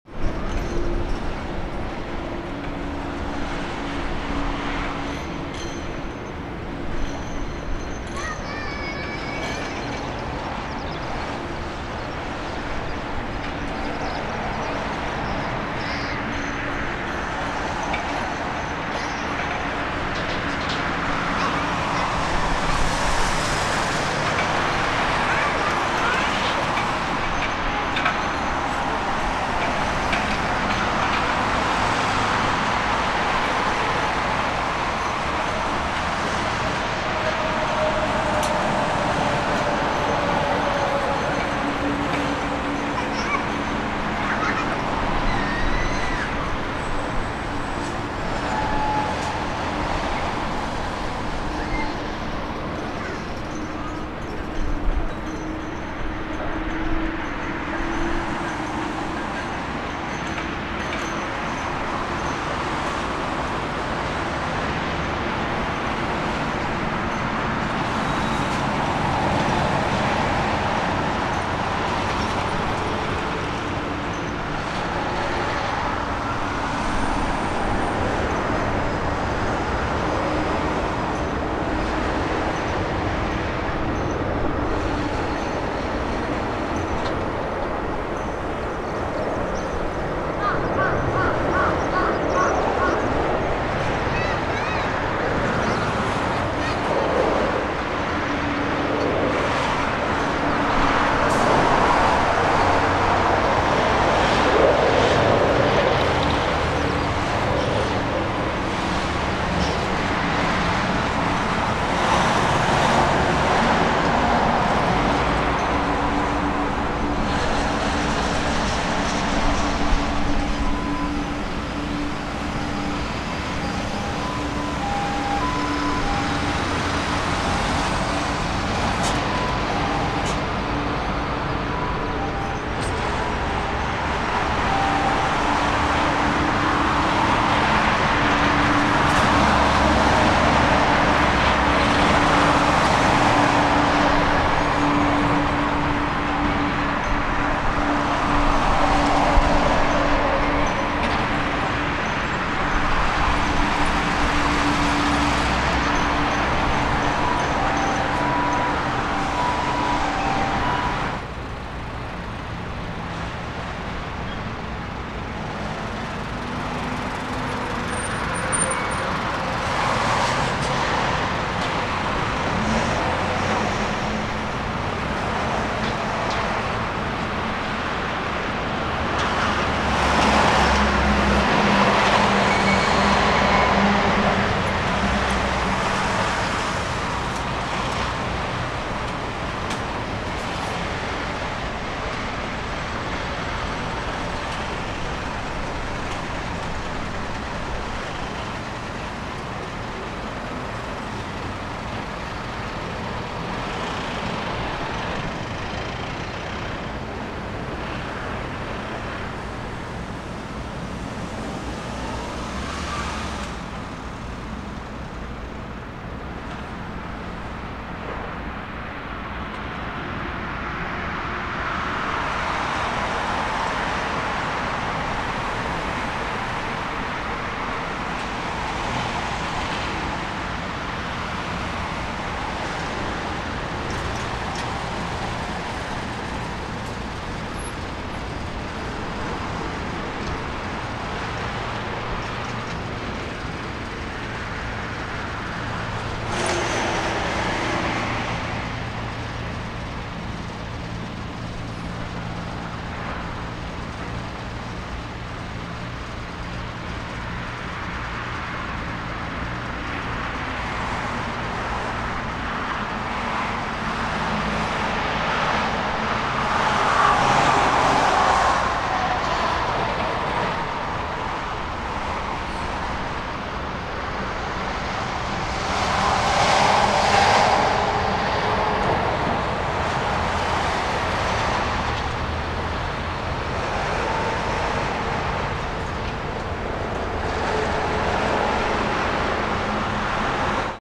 ✈[HD] ソラムナード羽田緑地で見るメラメラの中A350が2機のrwy22下り(羽田空港)
※最大40倍で撮影。ランチェンしたのでロケハンがてらきたので一脚も三脚もなく手持ちのためぶれぶれです。
Vietnam Airlines VN-A894/Singapore Airlines 9V-SME.
以前からストリートビューの映像にこのベンチができているのが写っていたのですがロープが張ってあり立ち入り禁止の看板も。やっと4月1日にオープンの記事が。
大田区の告知
南風の時にしかrwy22下りはないので夏場のポイントでしょうか。新ルートではrwy22上がりを20回/時間想定していますので大型機の場合はかなりの迫力になるのかな、04下りがあれば最高のポイントなのだが新ルートでも設定されていないようですね。
Apr.18 2019
羽田空港 ソラムナード羽田緑地ゲート１ベンチにて。